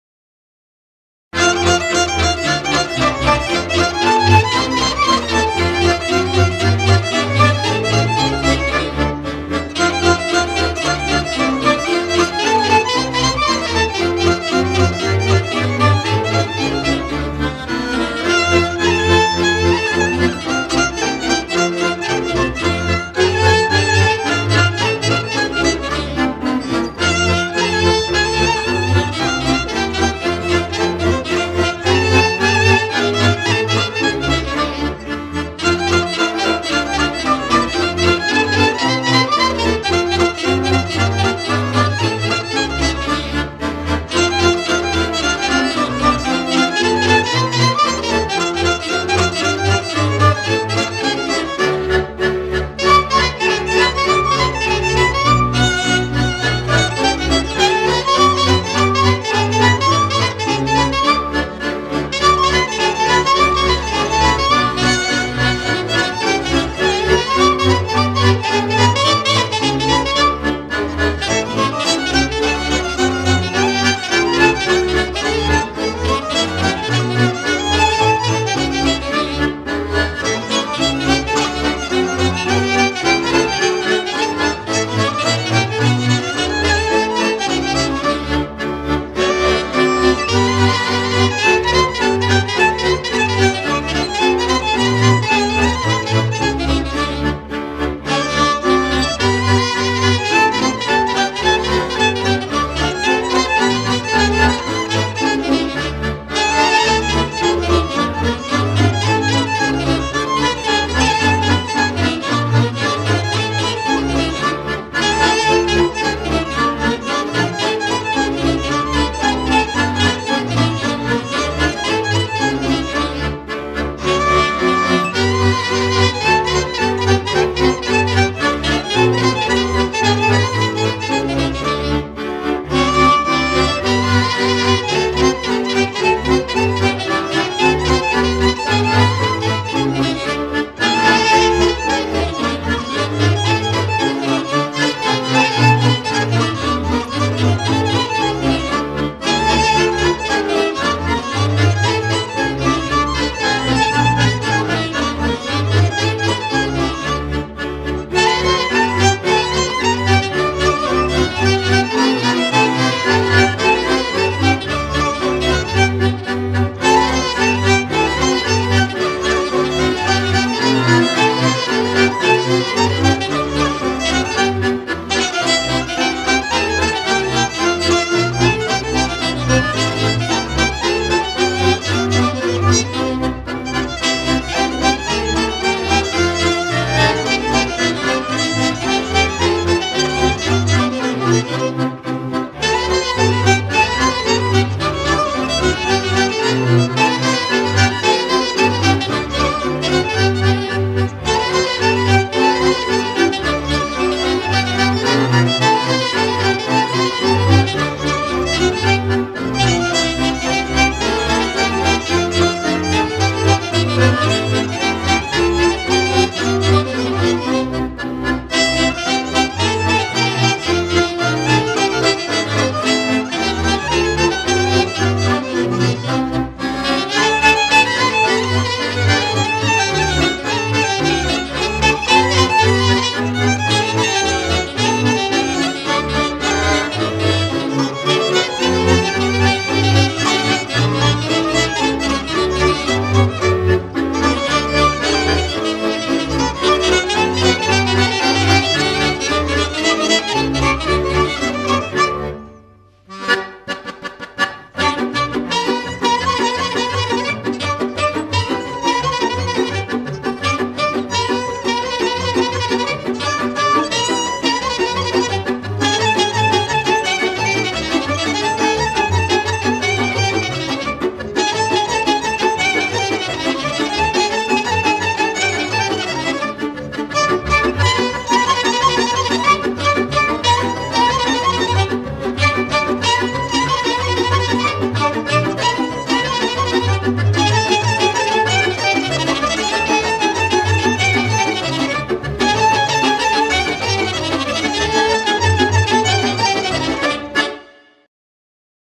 Romanian Traditional Dance - Transylvania
Traditional Romanian dance from Transylvania performed by Ariesana-Fiii Motilor folk ensemble from Mihai-Viteazu and Turda.